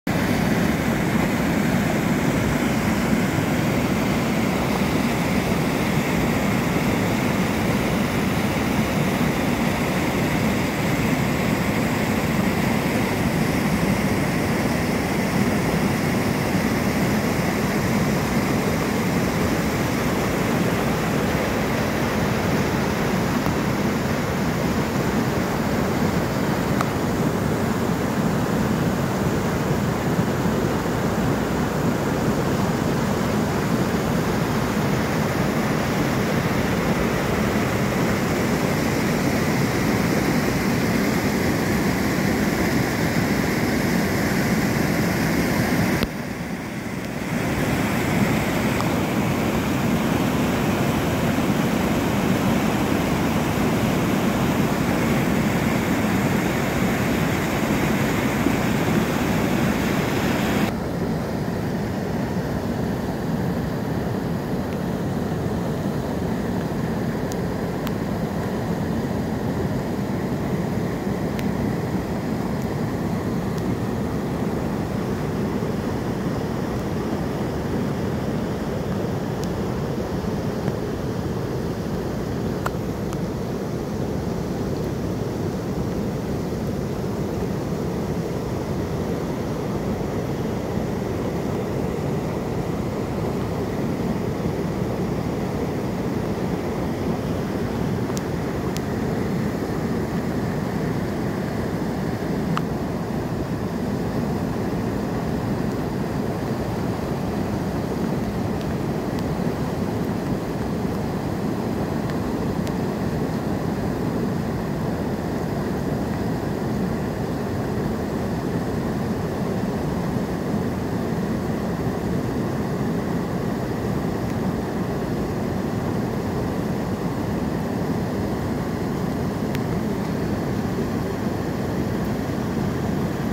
Řeka Úhlava, průtok vody u jezu. Zvýšená hladina 16. září 2024
Zvýšená hladina řeky Úhlavy. Stav v pondělí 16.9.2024. U jezu.